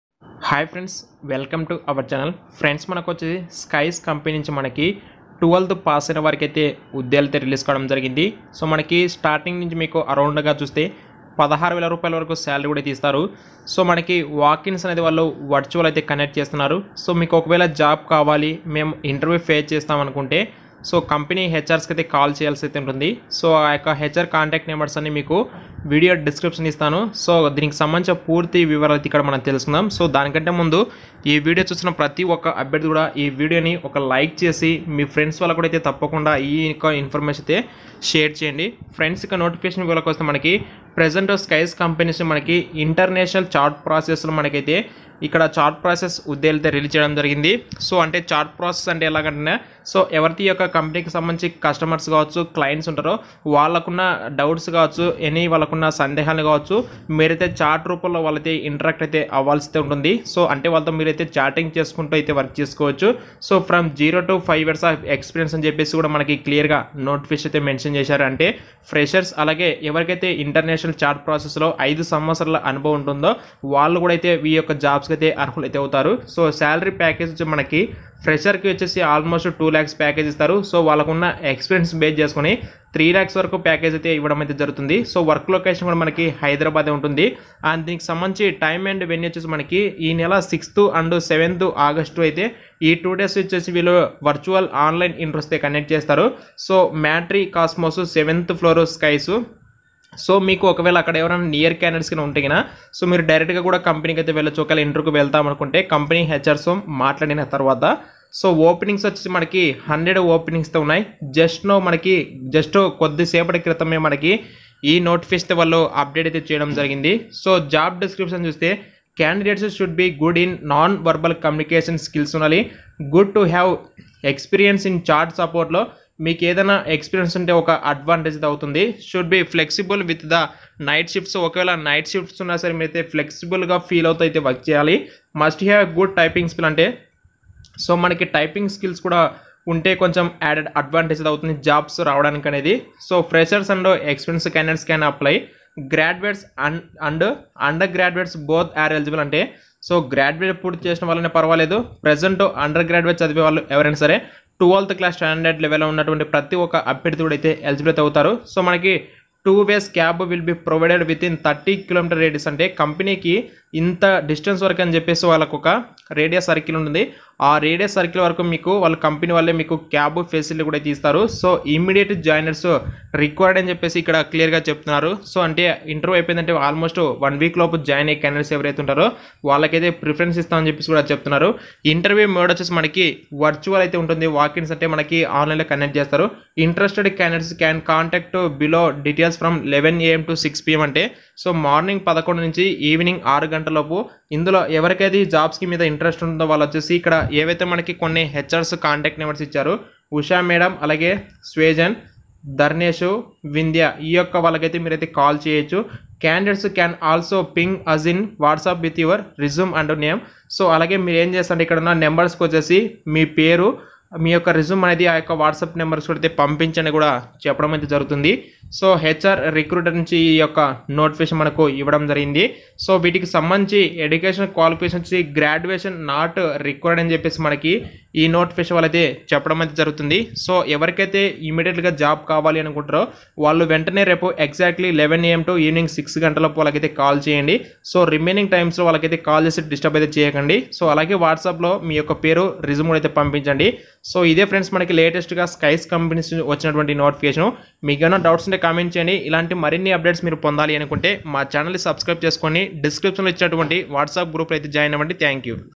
ఇంటర్ పాస్ చాలు 🤩 | Sykes లో ఉద్యోగాలు | Sykes Jobs Recruitment 2022 | Private Jobs
Interview Mode: Virtual - walk ins

Interested candidates can contact below details from 11am - 6pm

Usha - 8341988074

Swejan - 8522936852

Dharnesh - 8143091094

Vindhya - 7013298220

Candidates Can also ping us in whatsup with resume and name